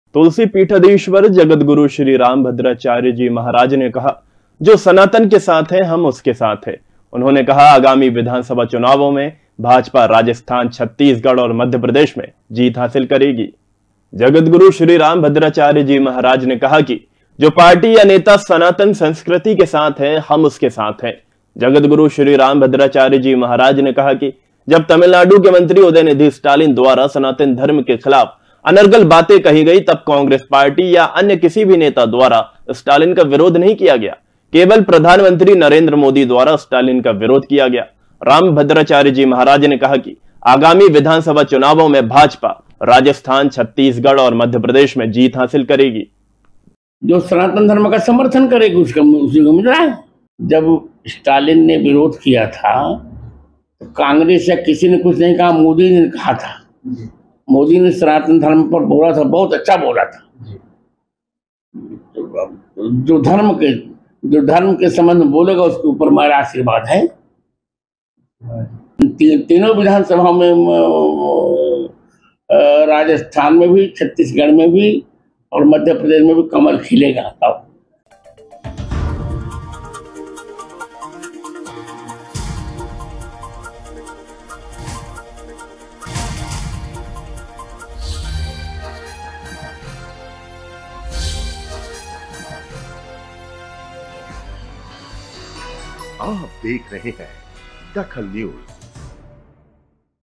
DAKHAL NEWS रामभद्राचार्य जी बोले बीजेपी जीतेगी
रामभद्राचार्य जी बोले बीजेपी जीतेगी
जो सनातन के साथ,हम उसके साथ